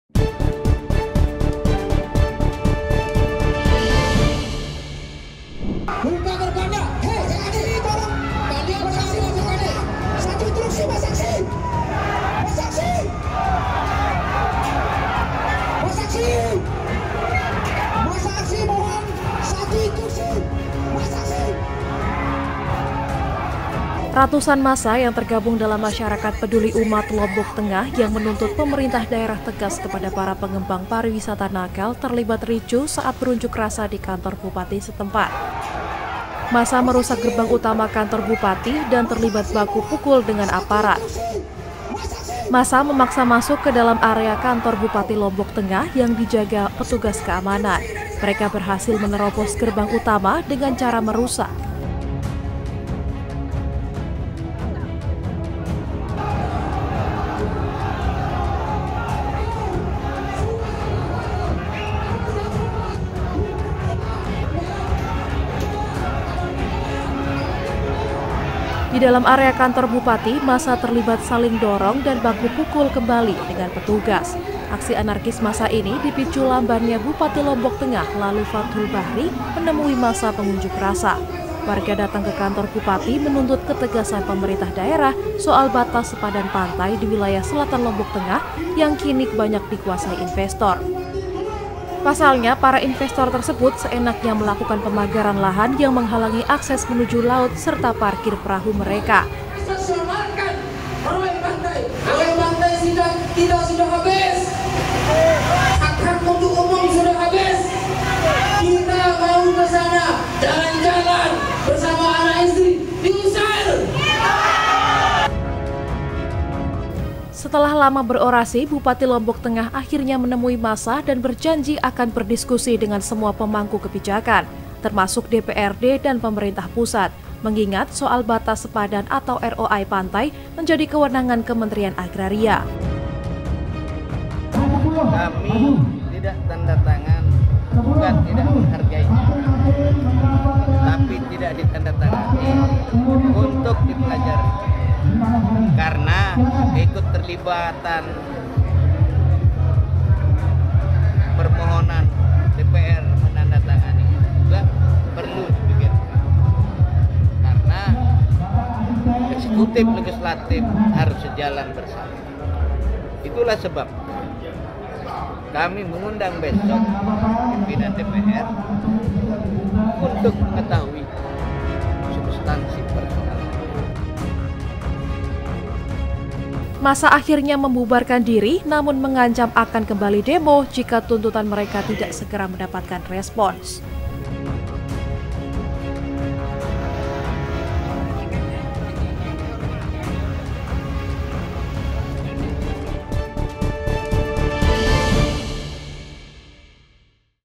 Demo Tuntut Pemerintah Daerah Tindah Pengembang Pariwisata Nakal, Massa Ricuh | OneNews Update
OneNews Update, - Demo Tuntut Pemerintah Daerah Tindah Pengembang Pariwisata Nakal, Massa Ricuh | OneNews Update

Ratusan massa yang tergabung dalam Masyarakat Peduli Ummat, Lombok Tengah, yang menuntut pemerintah daerah tegas kepada para pengembang pariwisata nakal, terlibat ricuh saat berunjuk rasa di kantor bupati setempat. Massa merusak gerbang utama kantor bupati dan terlibat baku pukul dengan aparat. (REG01) (RTR01)

AHM01
NU01